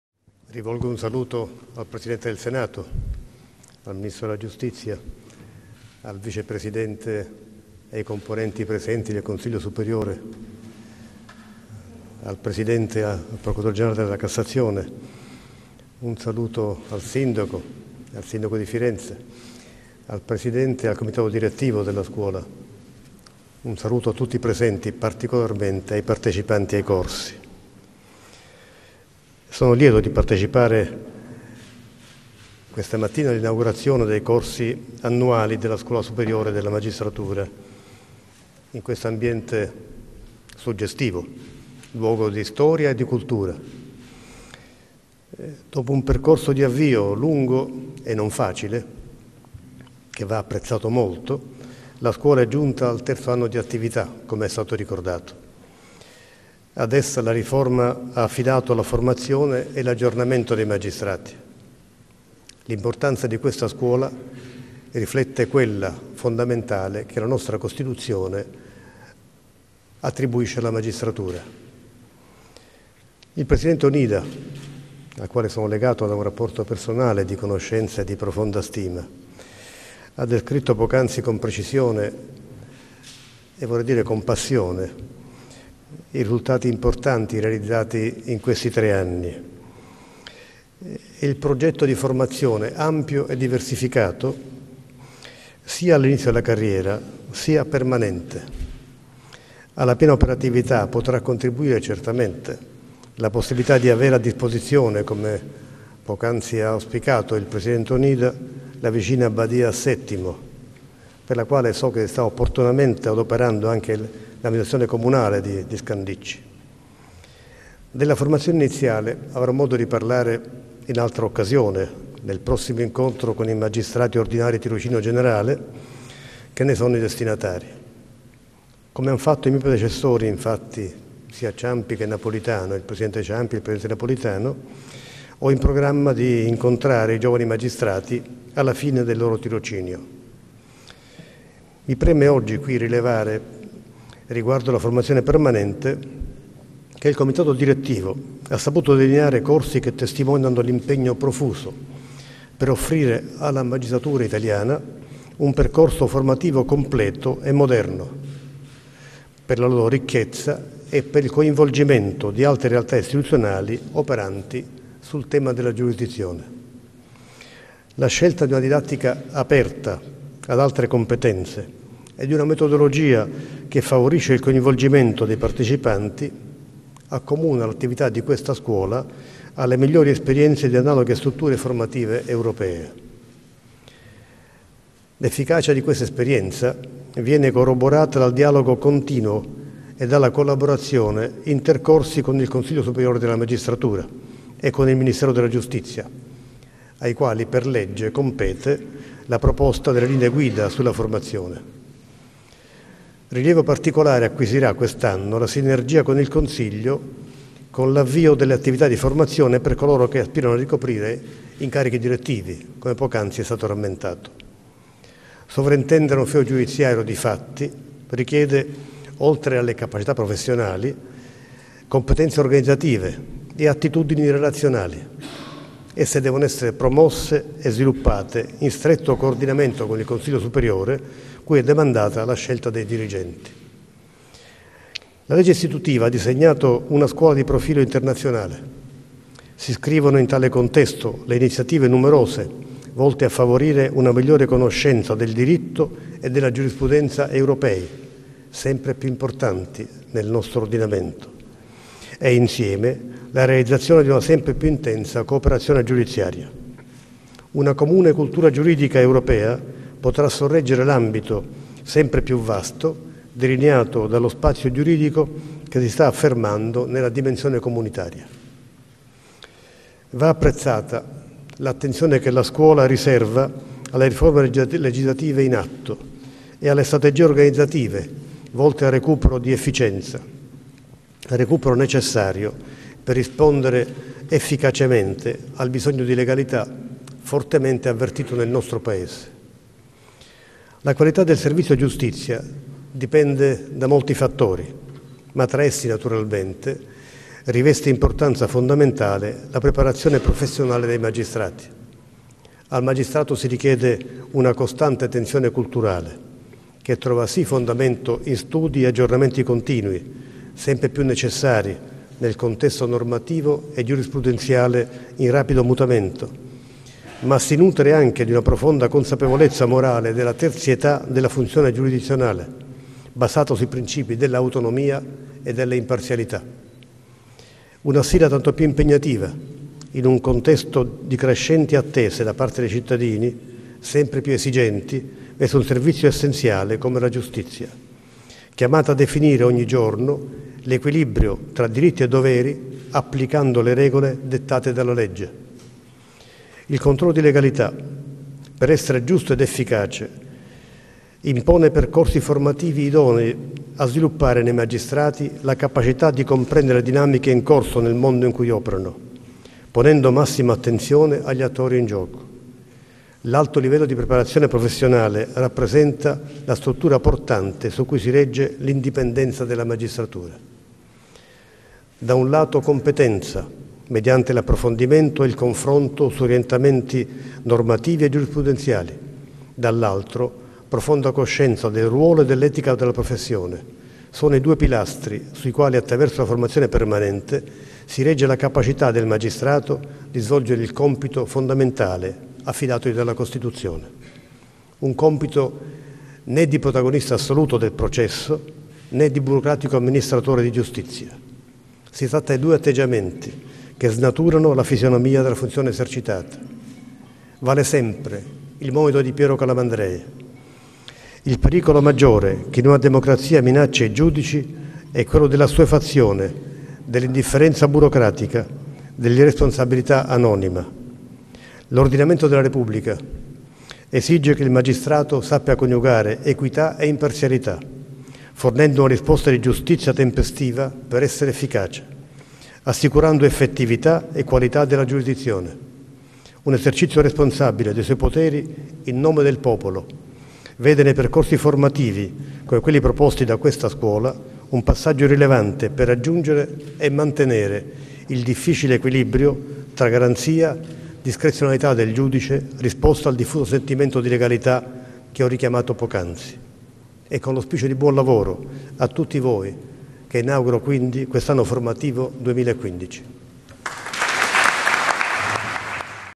24 02 2015 MATTARELLA NEL PAESE C'E' FORTE BISOGNO DI LEGALITA'
La magistratura deve darsi ‘strategie organizzative volte al recupero di efficienza’ per rispondere ‘efficacemente al bisogno di legalita’ fortemente avvertito nel Paese’. Lo ha detto il presidente della Repubblica Sergio Mattarella, a Scandicci per
l’inaugurazione dei corsi della Scuola della magistratura.